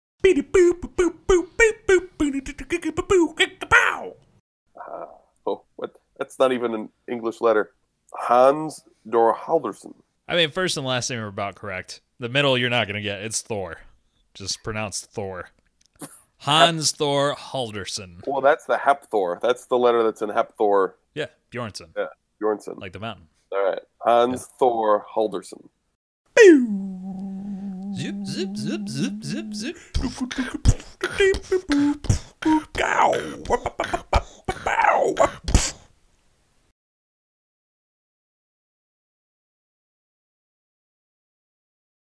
How to Pronounce: Hannes Þór Halldórsson
[Hans Thor Hall-door-son]

Hannes Þór Halldórsson is an Icelandic footballer and filmmaker. He is most recently known as the man who blocked Lio Messi's PK in the World Cup.

He was an assistant editor on the documentary Hrafnhildur- heimildarmynd um kynleiðréttingu, about the Icelandic boy Halldor who believed from a young age that he was a girl.